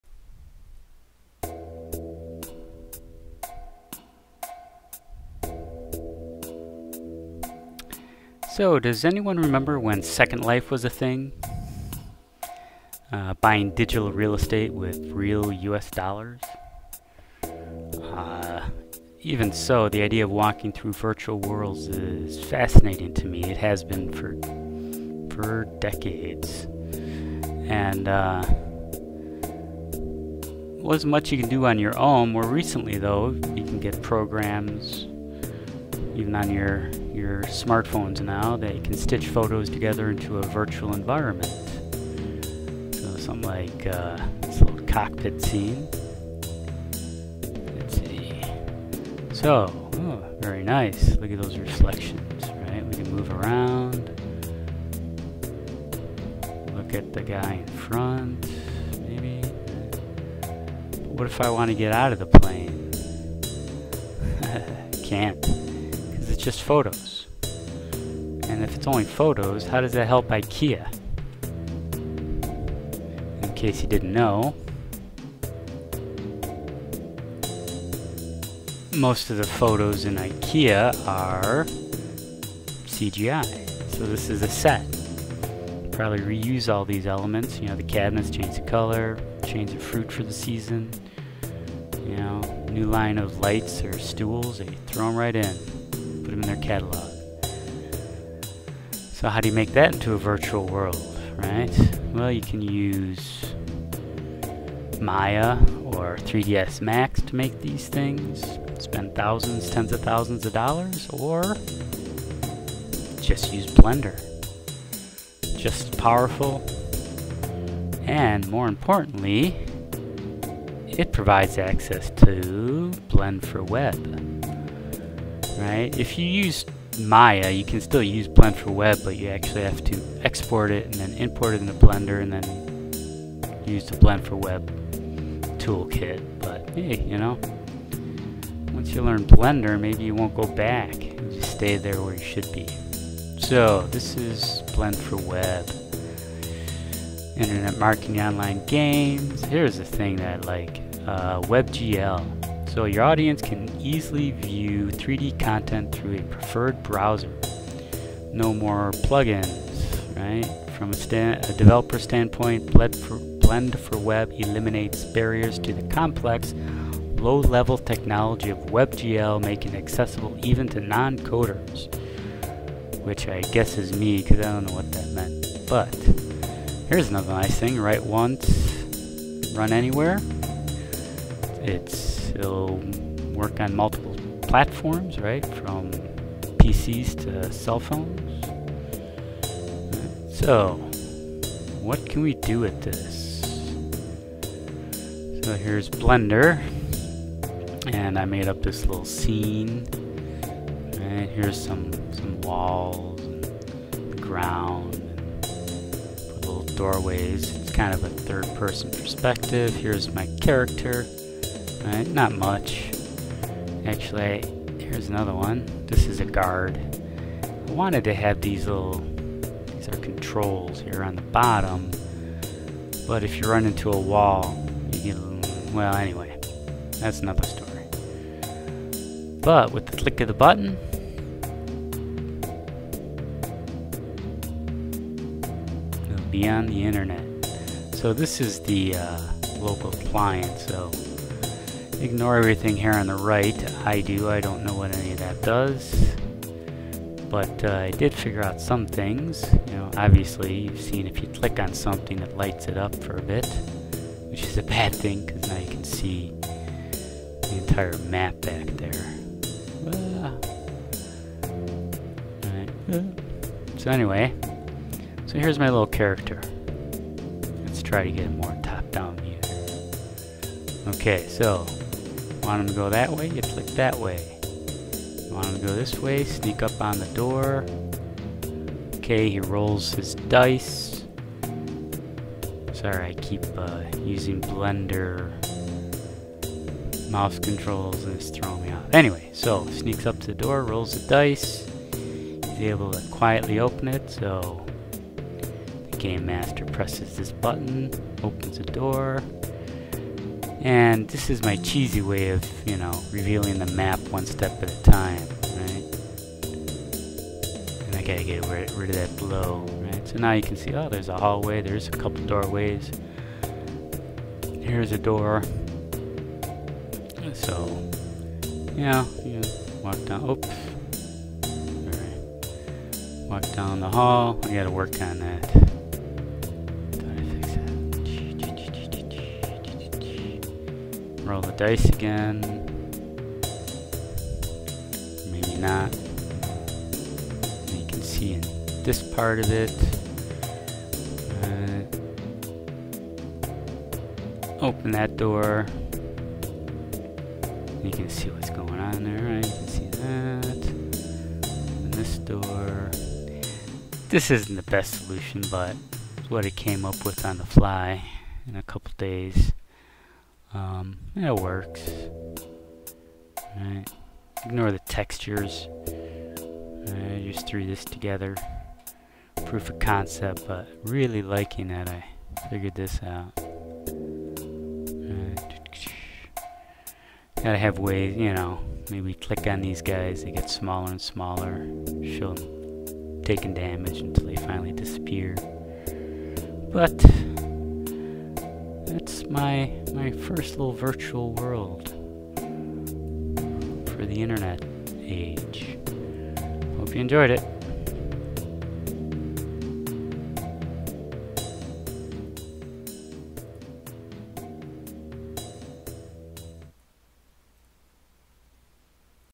Interactive WebGL and Blend4Web
Finally beginning to get a handle on Blend4Web, and (sort of) have it working. By that I mean, it runs on my local machine, but I have not yet tackled the next mountain - making it live somewhere on the internet.

Please excuse the popping from my poor mic positioning. Luckily I did not have to use a lot of words with the "b" sound in this video.

I adapted the floor plan from a dungeon adventure by the EveN Gang to make this scene.